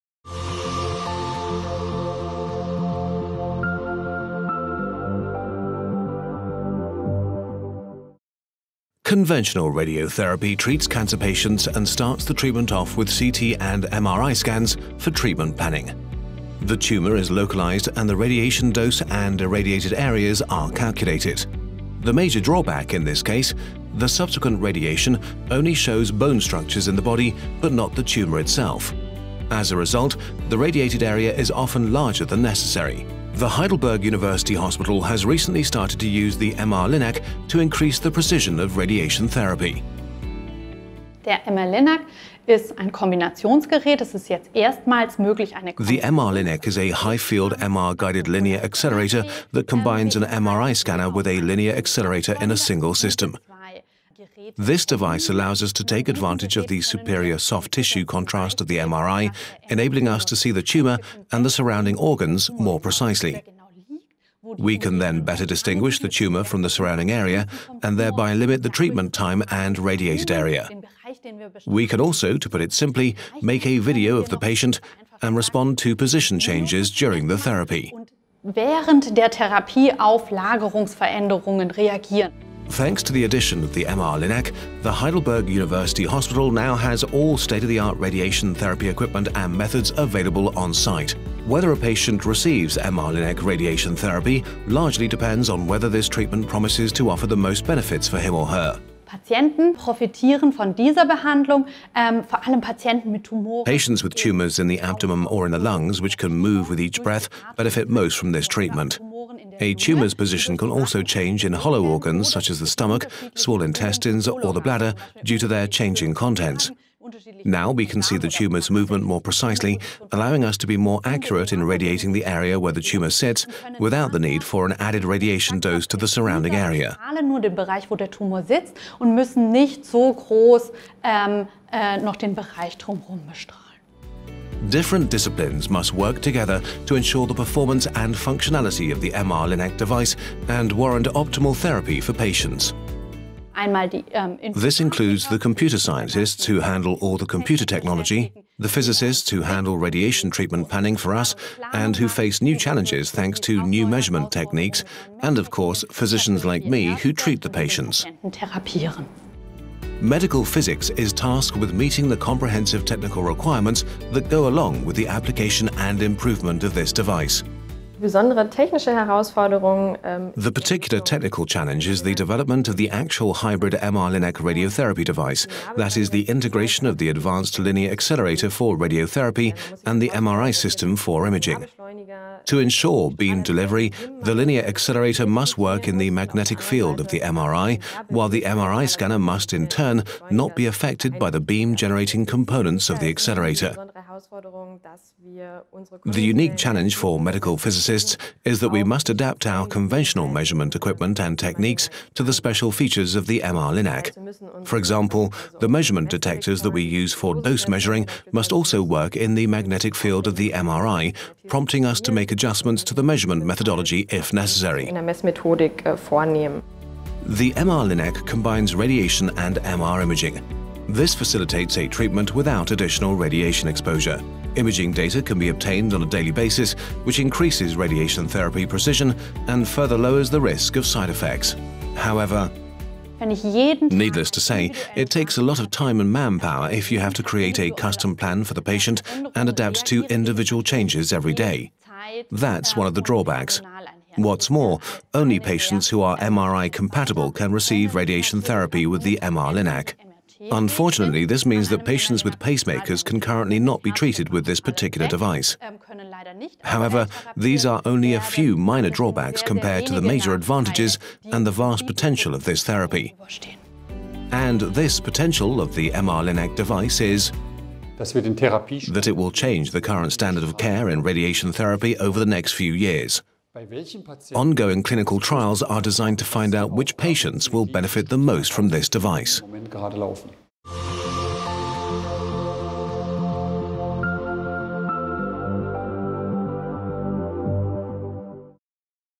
Where imaging and radiation meet – Radiotherapy with the MR-Linac
In conventional radiotherapy, the tumor is first localized using CT and MRT images in order to calculate the irradiated areas. The major drawback in this case: the subsequent radiation only shows bone structures in the body but not the tumor itself. As a result, the radiated area is often larger than necessary. In our video you will learn how the MR-Linac can be used for more precise radiotherapy.